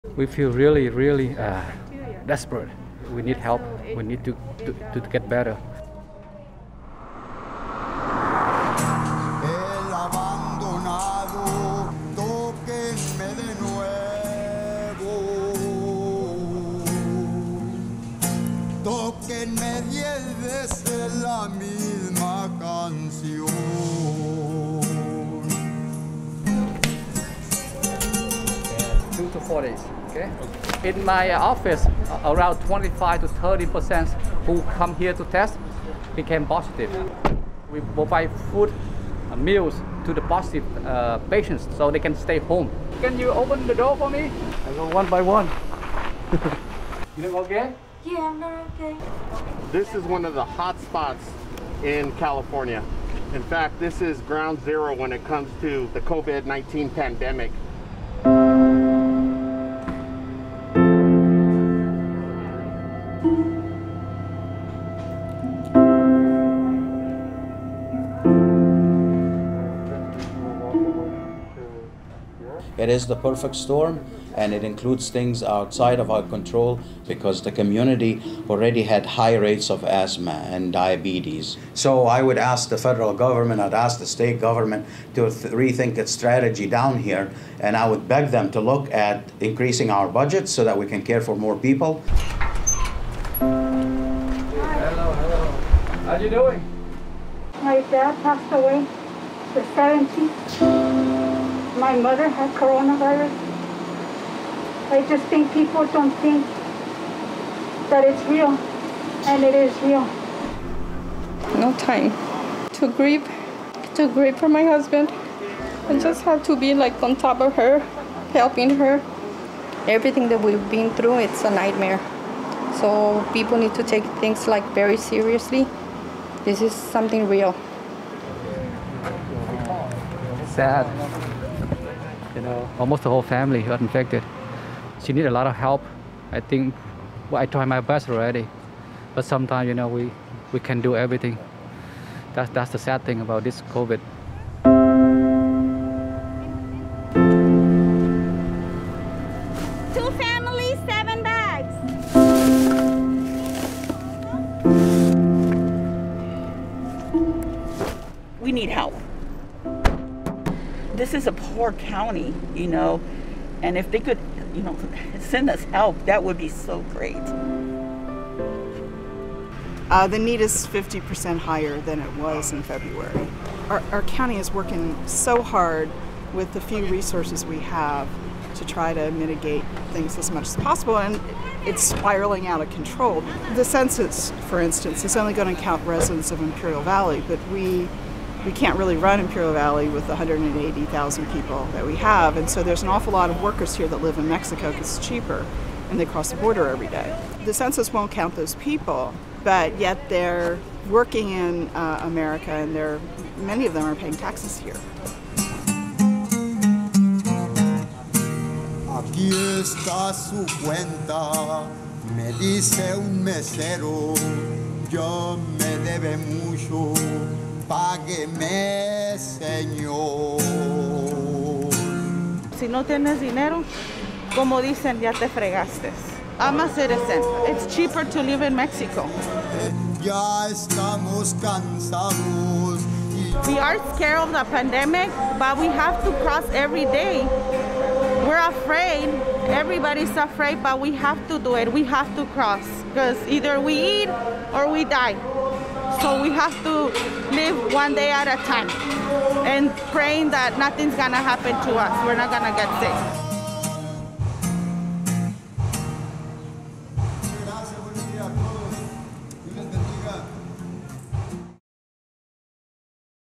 Neighboring Imperial County is 'Forgotten Frontier' in COVID-19 Fight
Just to San Diego's east along the U.S.-Mexico border, Imperial County has been ravaged by the coronavirus pandemic.